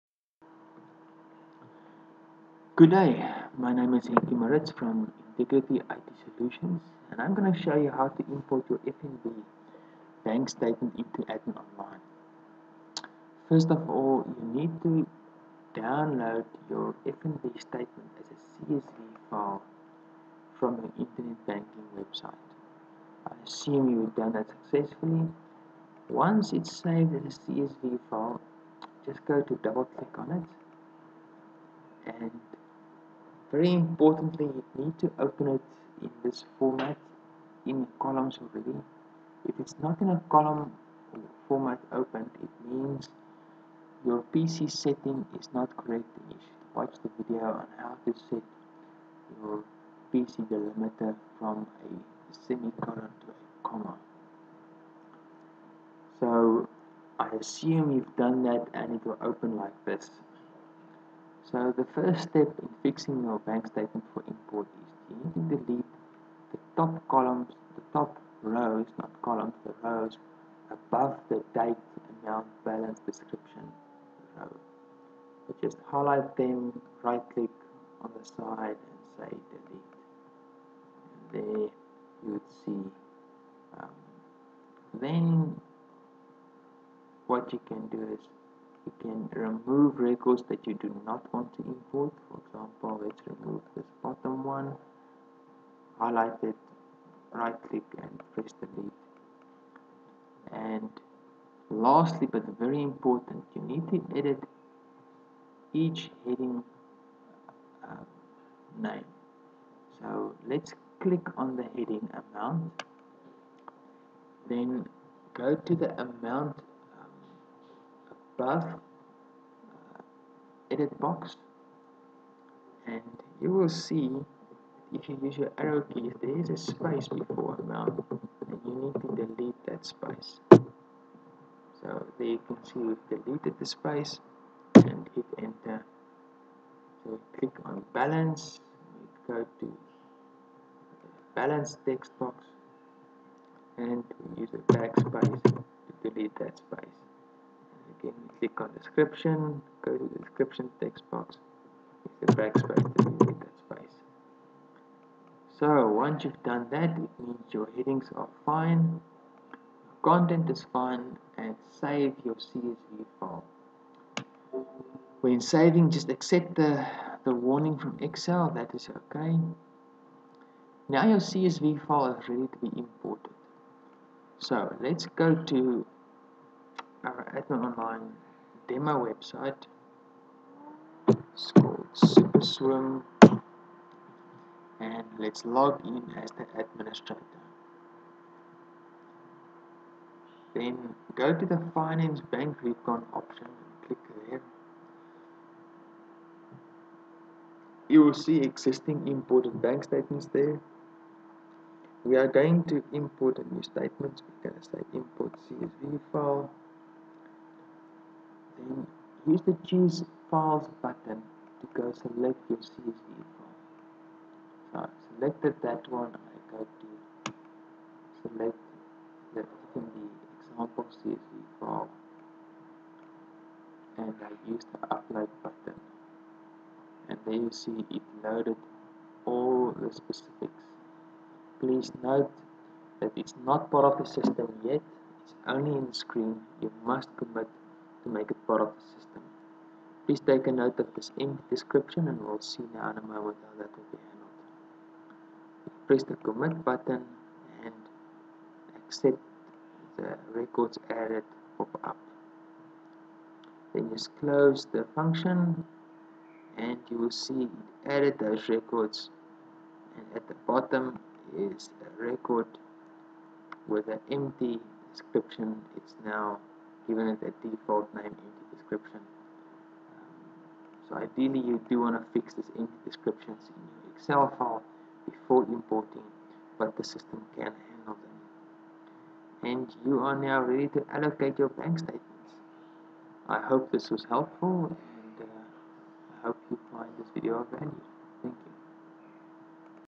AdminOnline How to Import FNB Bank Statement (in English)
Show AdminOnline customers how to import a FNB bank statement into the Bank Recon function.